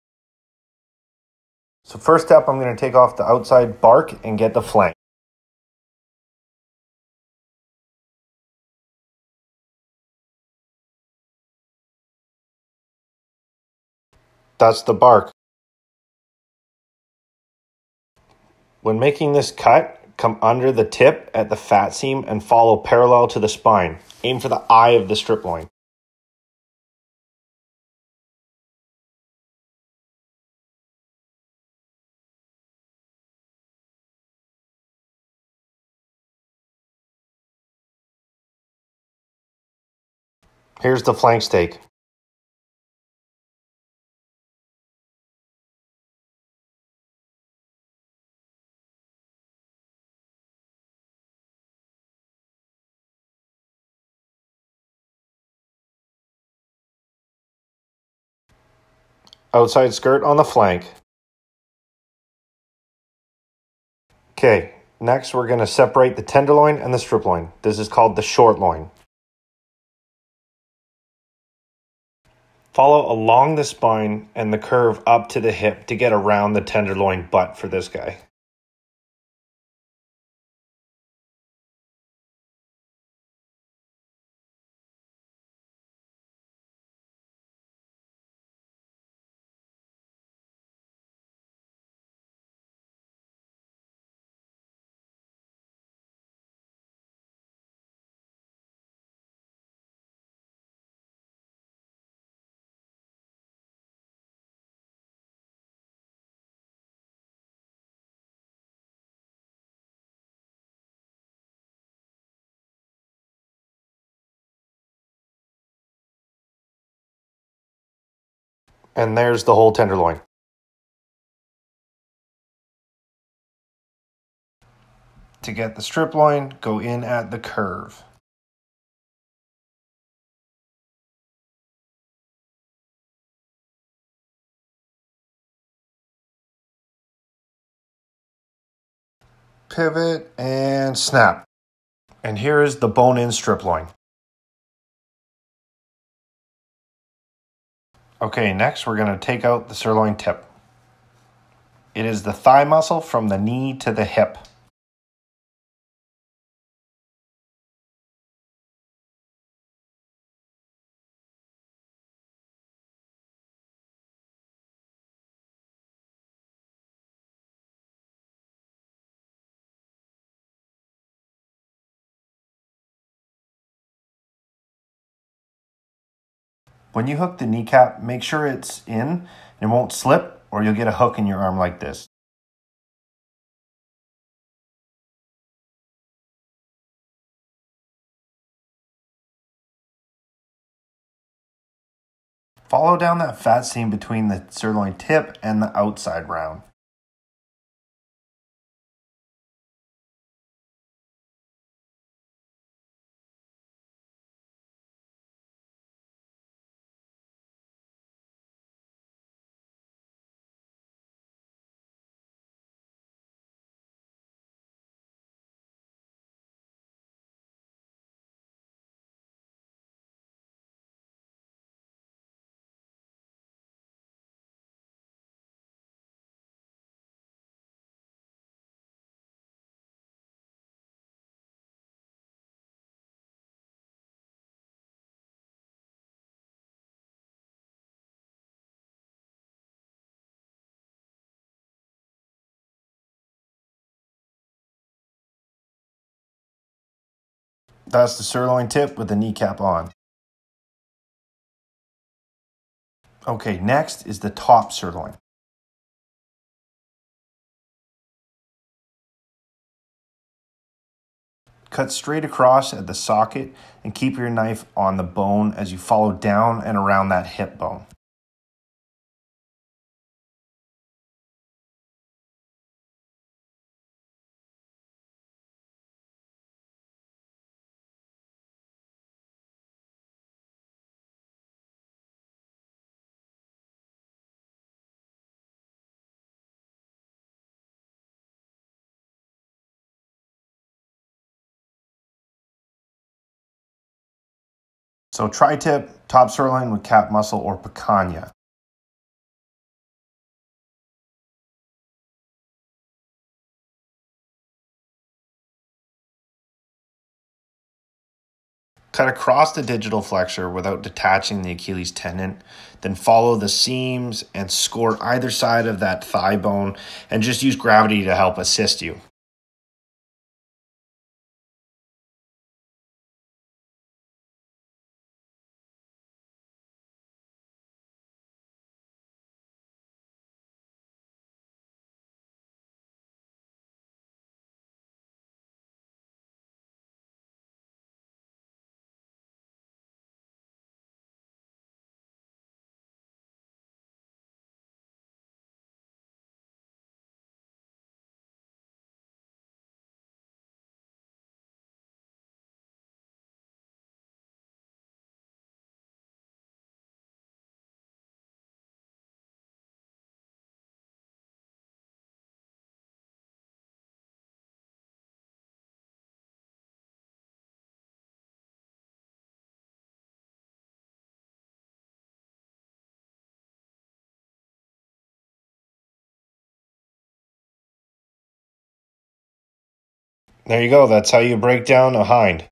How to butcher a beef hind quarter: step by step
In this video I'm going to show you guys how to breakdown (or rail cut) a whole beef hind quarter. I'll tell you what each piece is, and how to properly process through this carcass.
Sorry for the bad kung-fu movie audio edit, I lost the audio on the original.